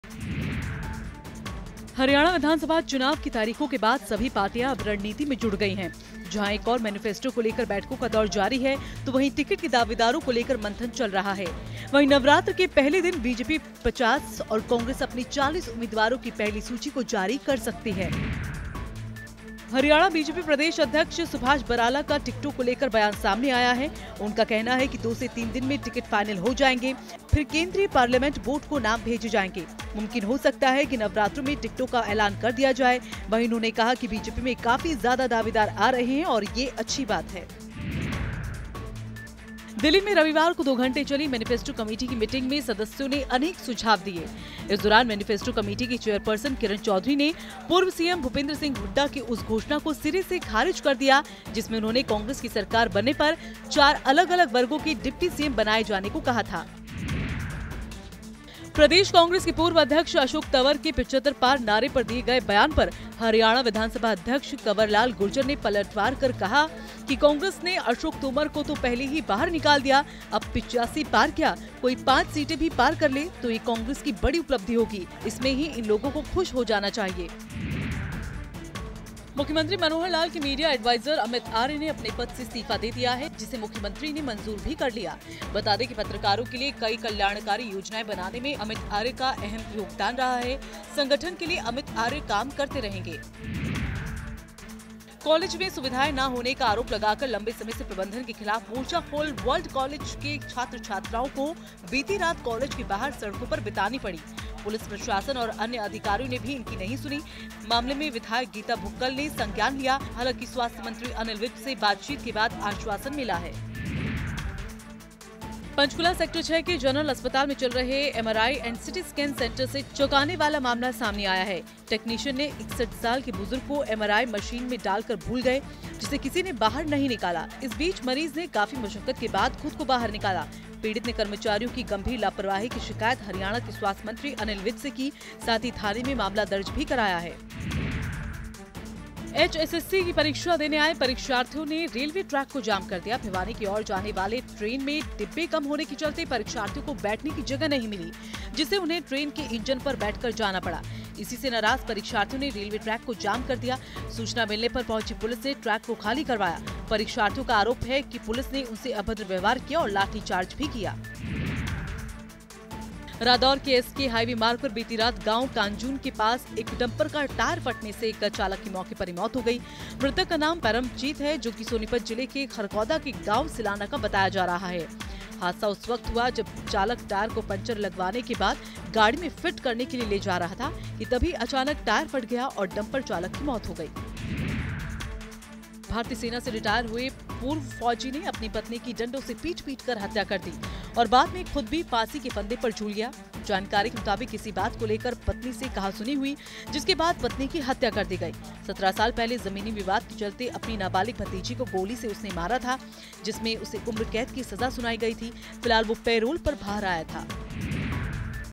नवरात्र में BJP-CONG की पहली लिस्ट !
BJP-CONG की नवरात्र के पहले दिन आ सकती है लिस्ट , CM के मीडिया एडवाइजर अमित आर्य का इस्तीफा, मेनिफेस्टो कमेटी की हुड्‌डा की घोषणाओं पर रोक...देखिए HARYANA TOP 10